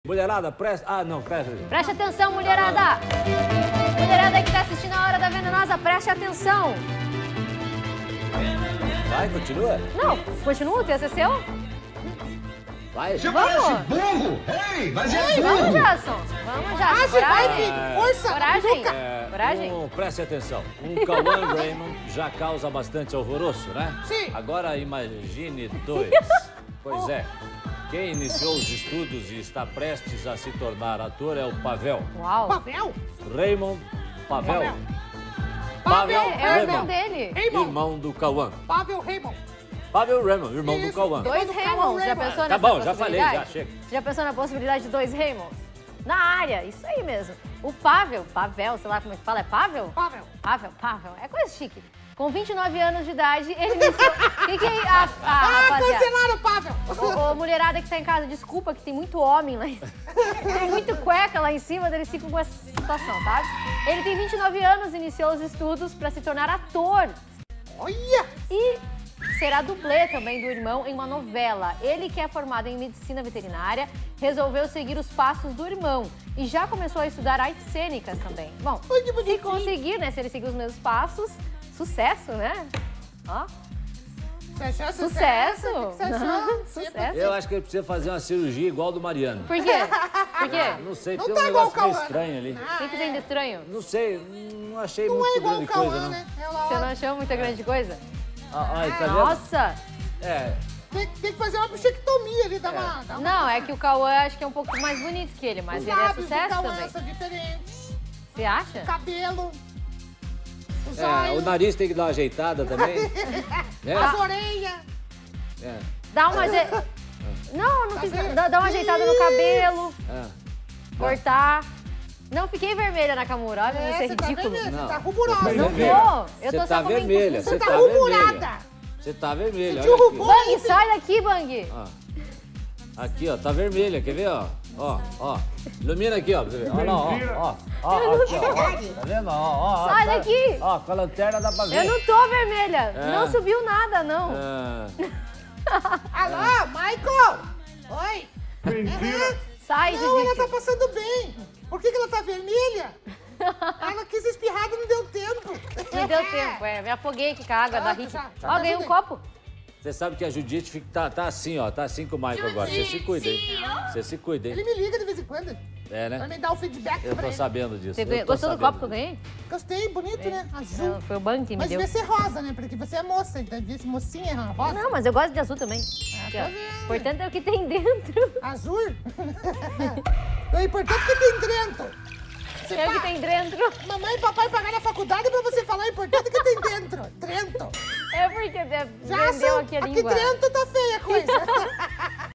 Irmão do ator Cauã Reymond larga medicina veterinária e se torna ator
Mulherada que se prepare pois logo logo teremos dois "Reymonds" na área. Veja!